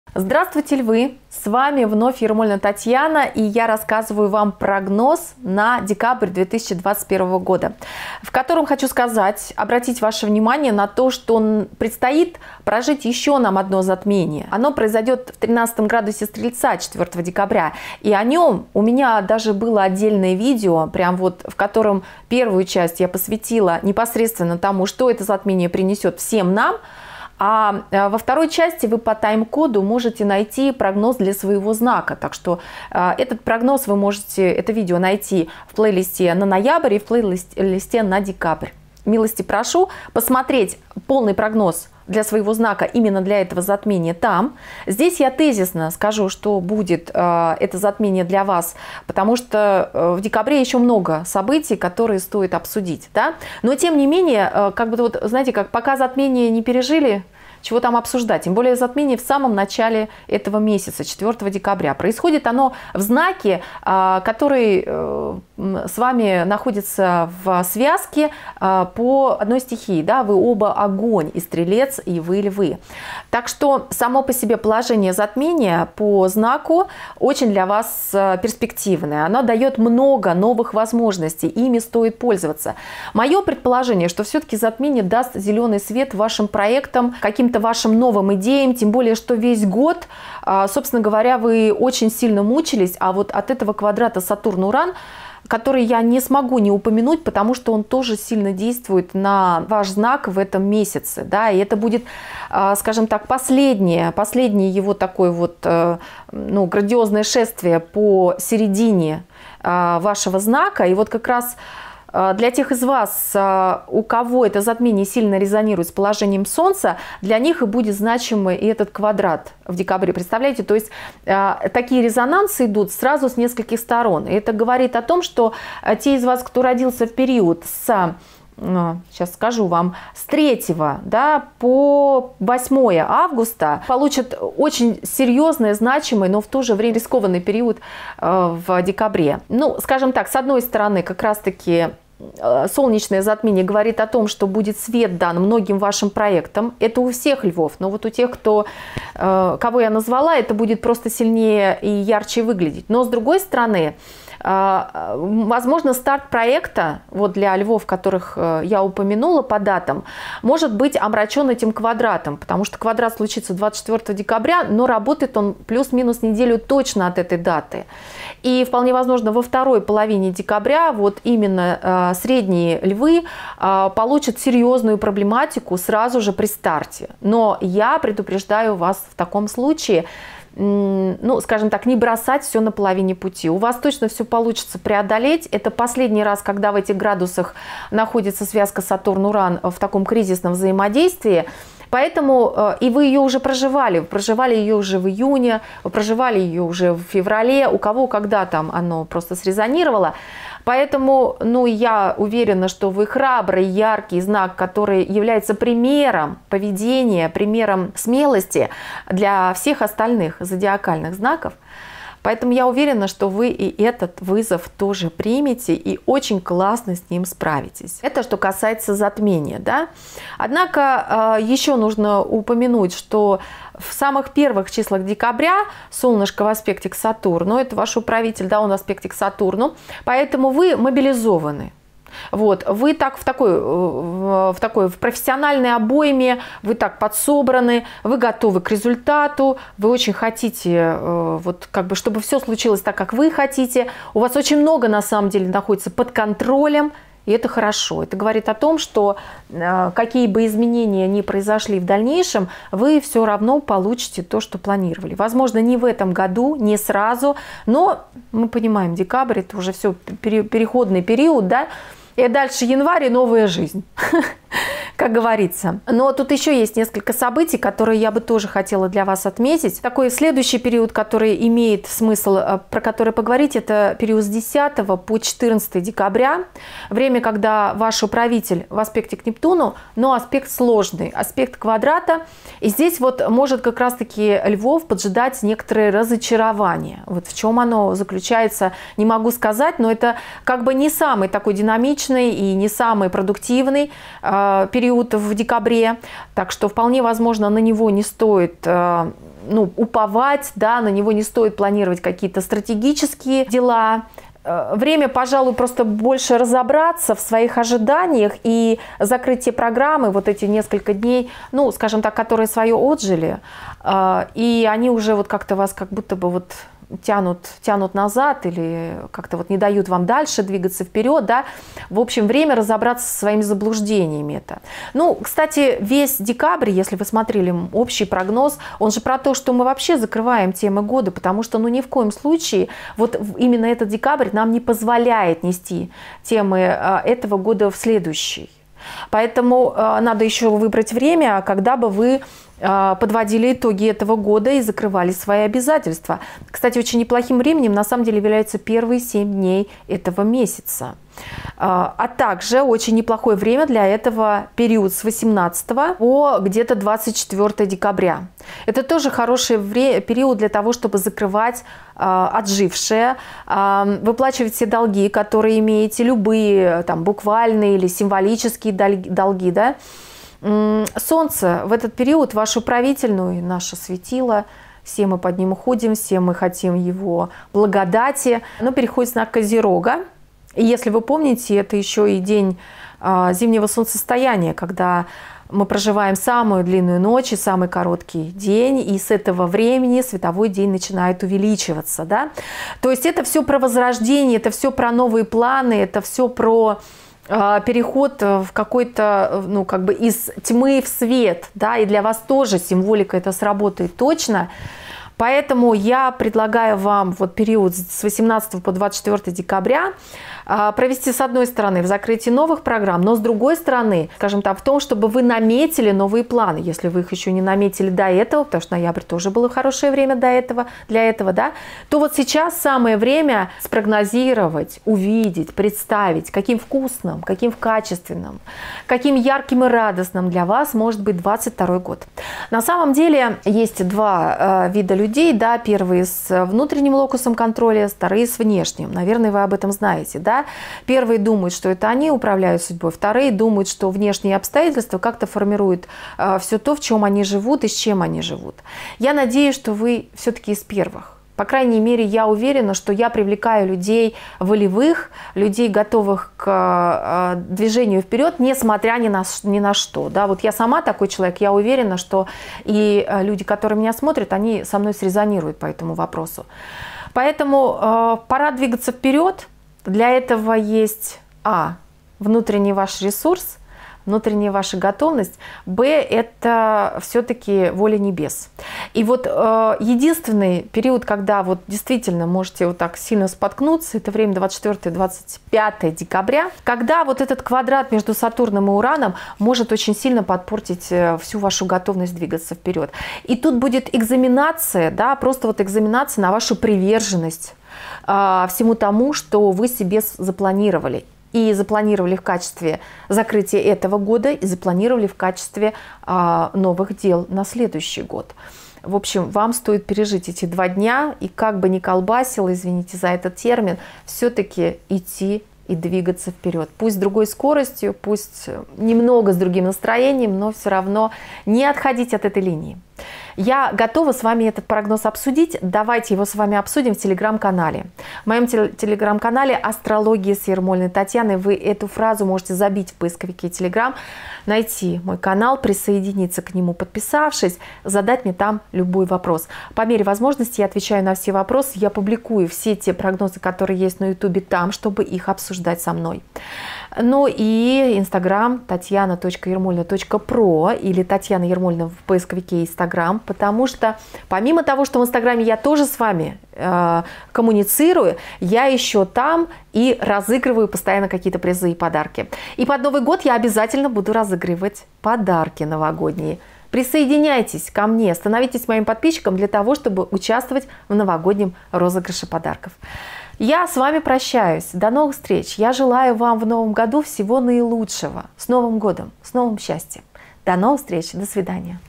ЧТО ПРИНЕСЁТ ОКОНЧАНИЕ ГОДА ЛЬВАМ. Прогноз на декабрь 2021г.
В этом месяце небесные события выстроились в очень плотный ряд, скучать не придётся никому. Но для одних небеса приготовили кучу задач и настоящие головоломки, другим обещают помощь в их делах. В моем прогнозе я рассказываю о том, что ожидает ваш знак.

Автор видео блога: астролог, сертифицированный коуч Ермолина Татьяна